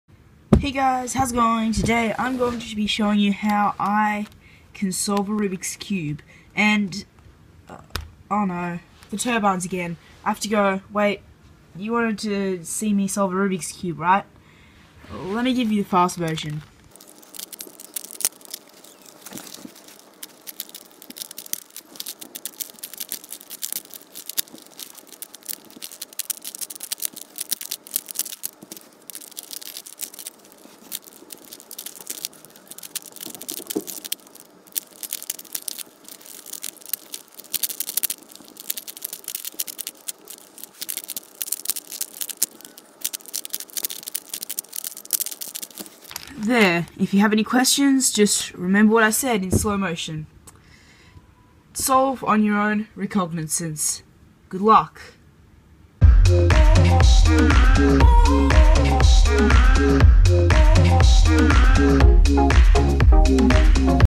Solving a Rubik's Cube!
I can now solve a Rubik's cube! It is a Rubik's brand puzzle and it is my dads.